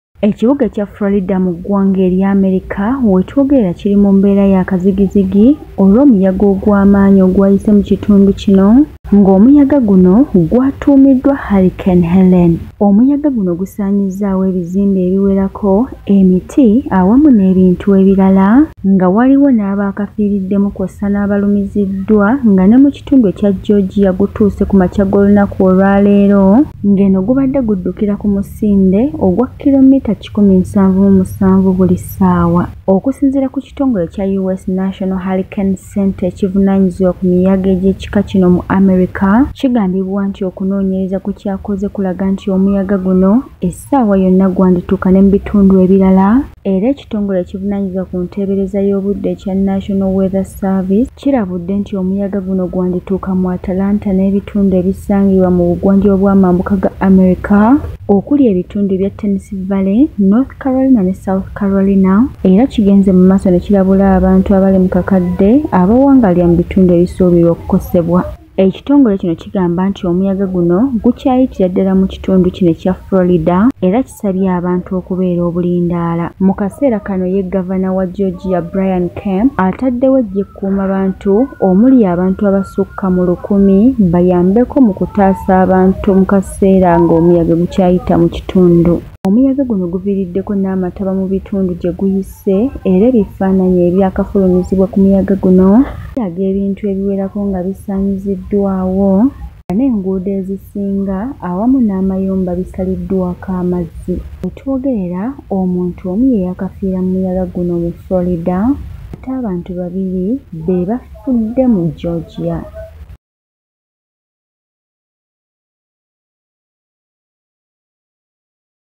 Hurricane Helene brings life threatening conditions as it reaches Georgia from Florida.
Streets in Georgia under water as deadly hurricane helene continues North.